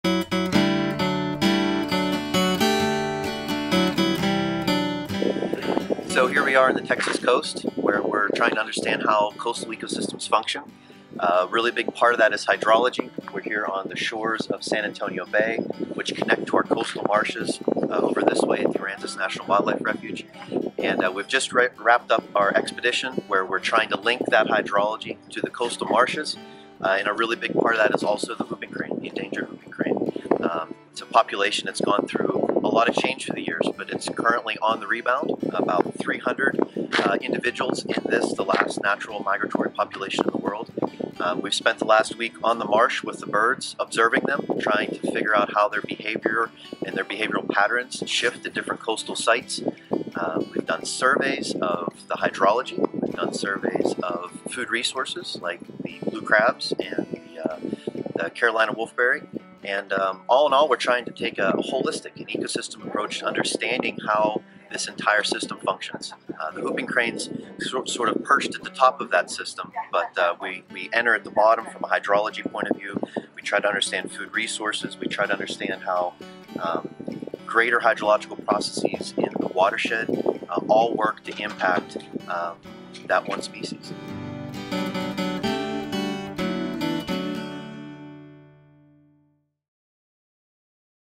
Protecting Whooping Cranes and Coastal Habitats in Texas
Join lead scientist Dr. Jeffrey Wozniak and his team of researchers studying the endangered Whooping Crane and the coastal marsh ecosystems these unique birds call their winter home.